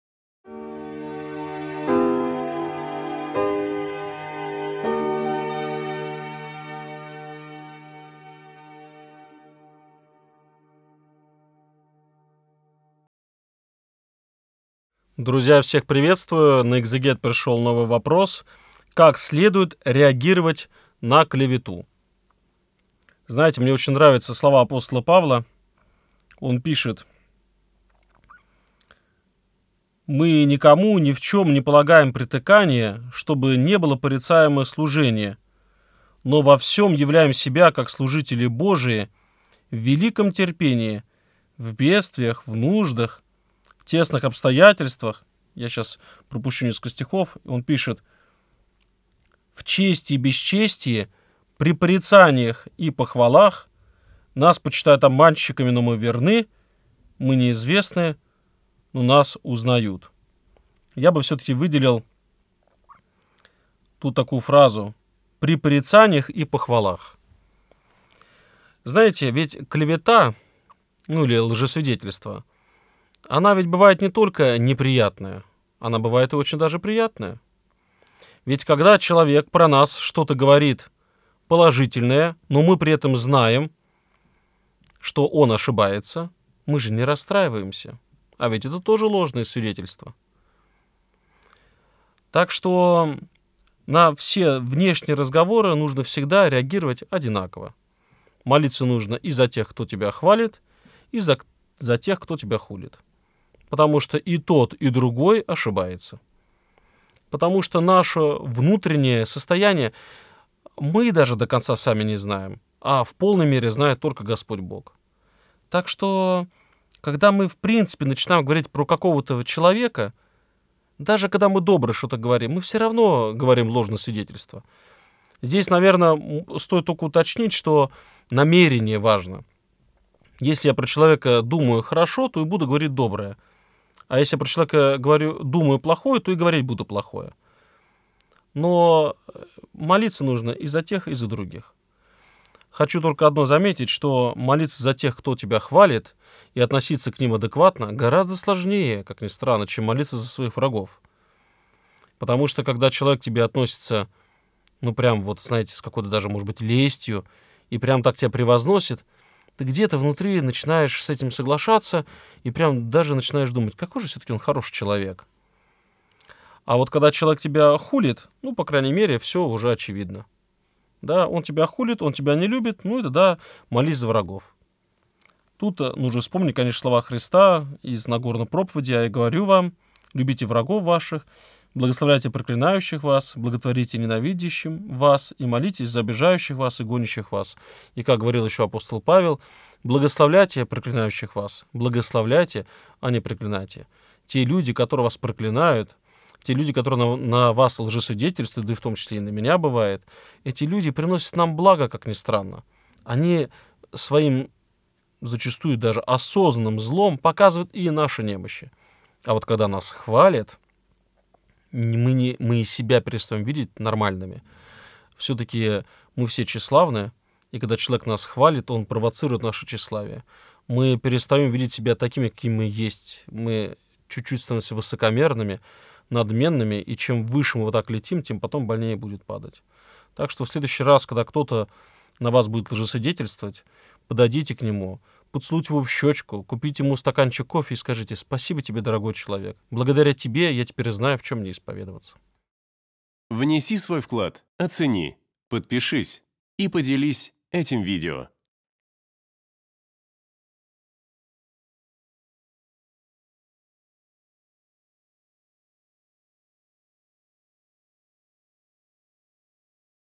Как следует реагировать на клевету ? Священник Павел Островский
В сегодняшнем видео вместе со священником Павлом Островским рассуждаем на тему: "Как следует реагировать на клевету?"

🔸 Порицание и похвала.
🔸 Молитва за осуждающих.
🔸 Христианская любовь.
🔸 Тщеславие души.